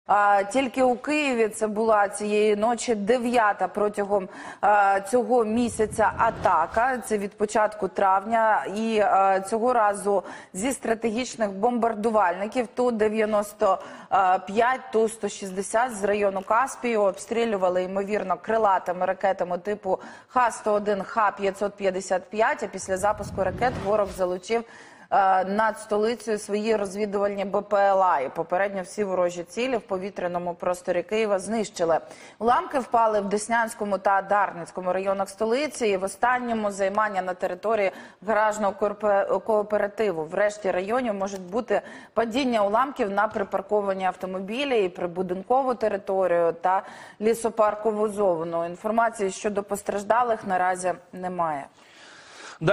Нічний обстріл Києва: уламки впали у кількох районах
Вночі 18 травня ЗС РФ здійснили масований обстріл Києва, випустивши у напрямку столиці крилаті ракети.